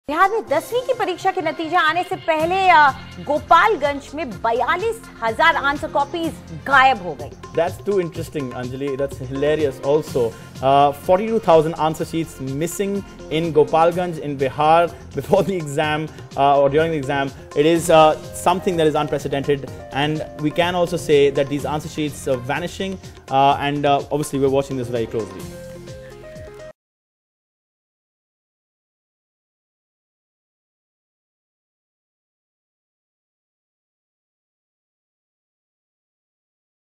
Bihar Board Class 10 Results Postponed After Thousands Of Answers Sheets Go Missing
The announcement of the Class 10 results of Bihar School Examination Board (BSEB), which was scheduled for today, has been postponed after thousands of answer scripts went missing. The result will be now declared on June 26, said the BSEB. The principal of a school  in Bihar's Gopalganj district was summoned by the BSEB and was later detained by police for interrogation.